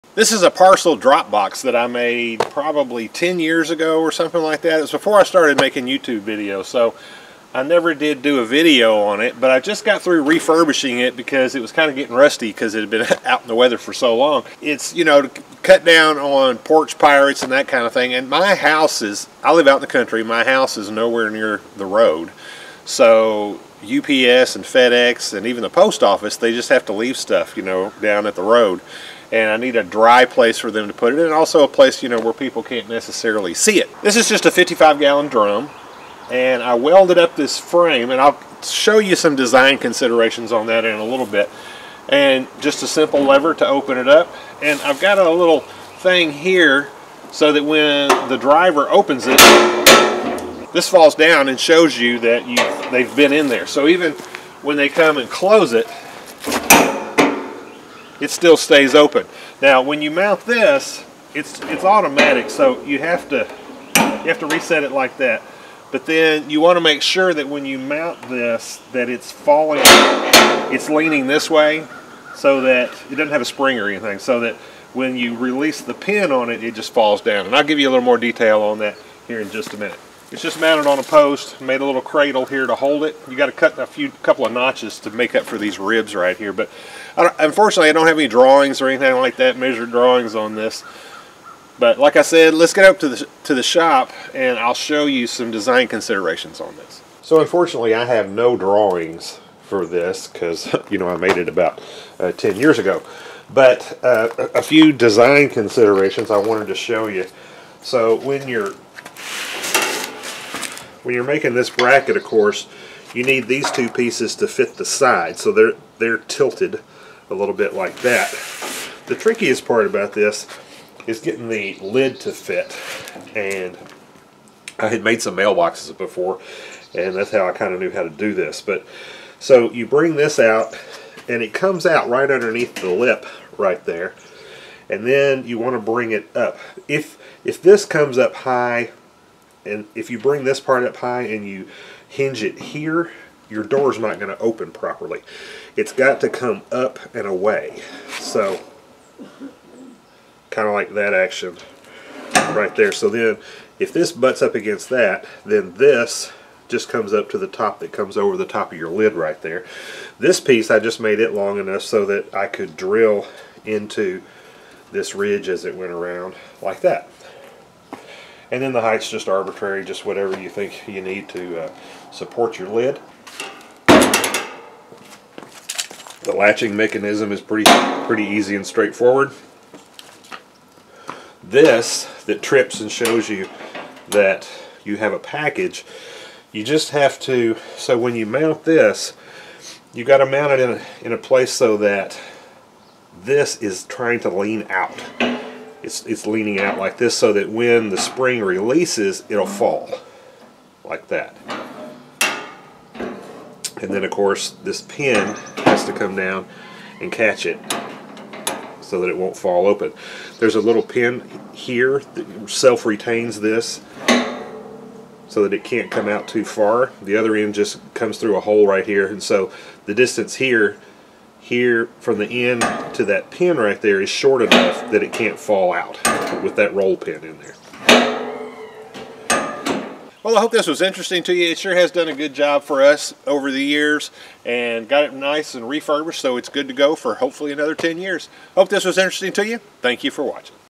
DIY Parcel Box
Showing a parcel box I built years ago and how to build it.